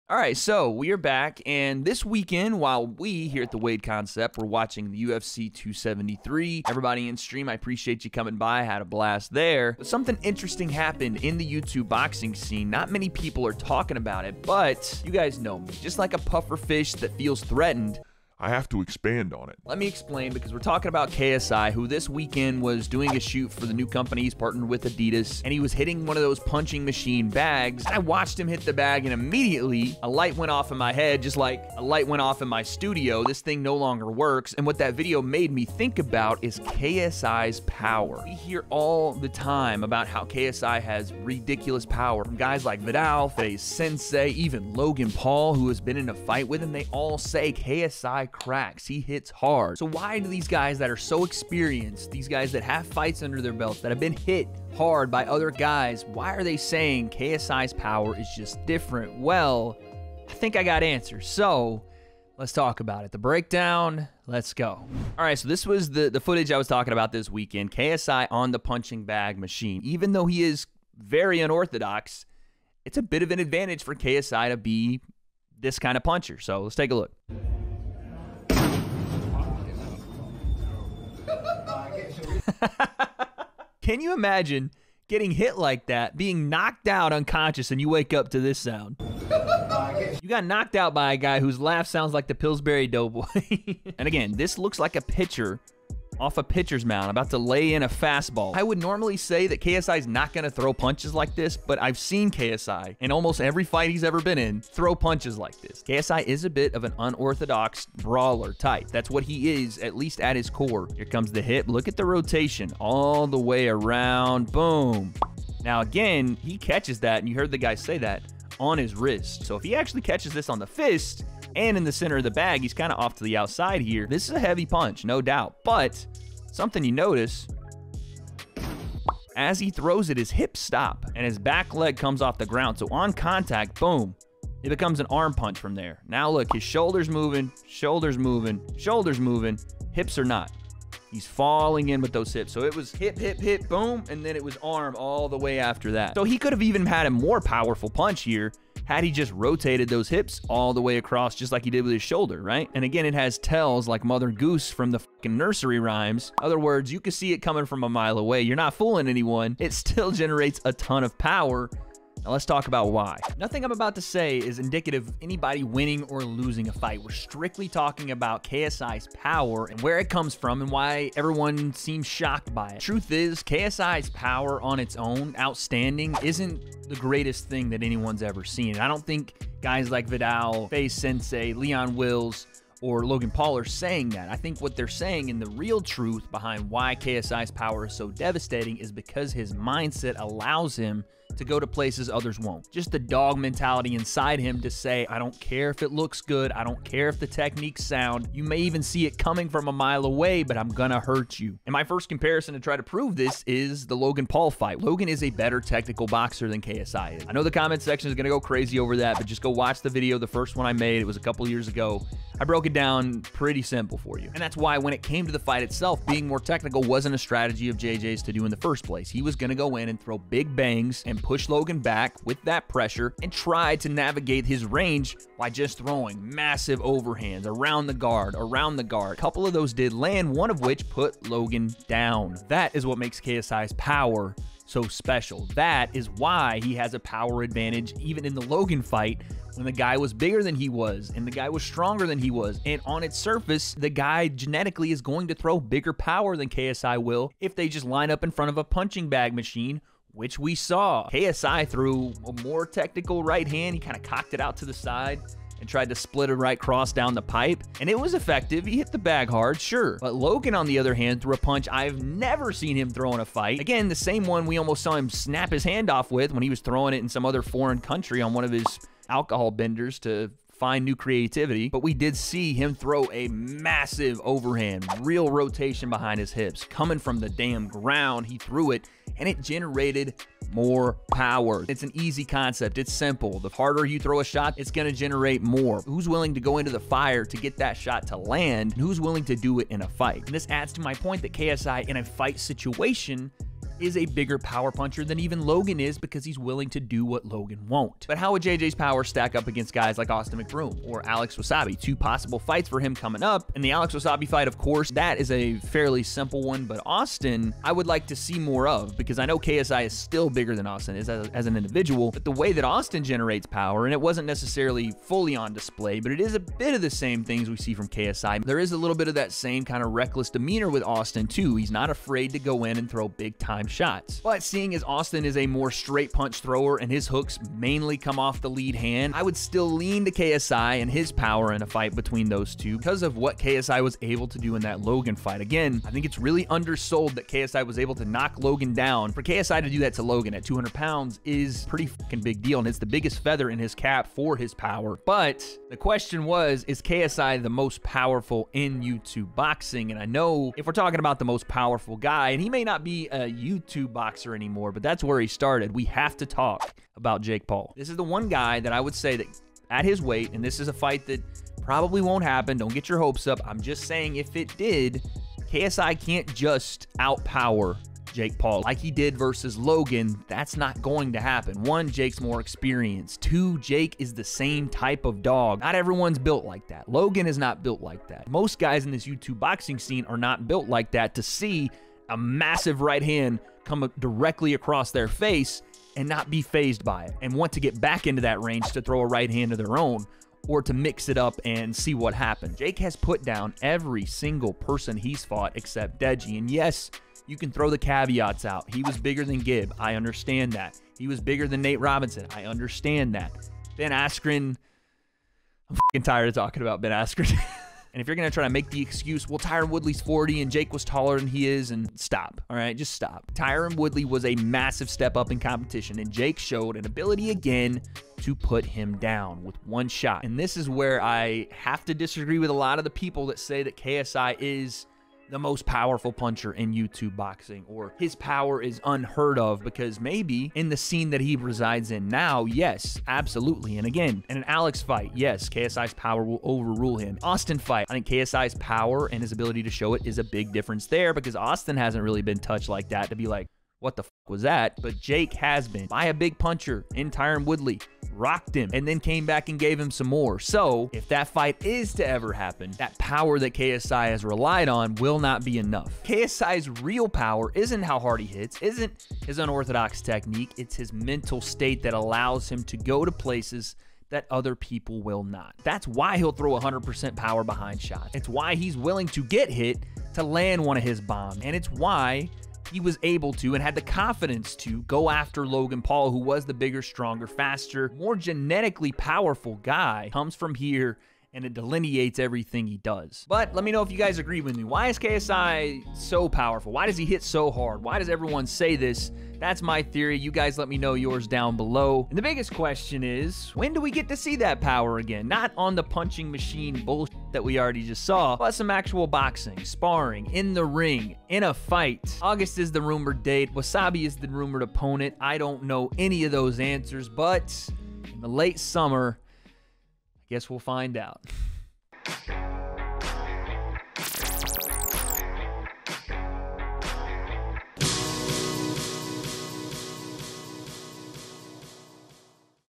THE TRUTH About KSI's "POWER"...
Is KSI's Power.. Overrated? Or the biggest WEAPON in his boxing skill tree?? Let's Debate!!

💥 FOLLOW THE W.A.D.E. Concept on Twitch!!

"The WADE Concept" 27758 Santa Margarita Pkwy Box 581 Mission Viejo, CA 92691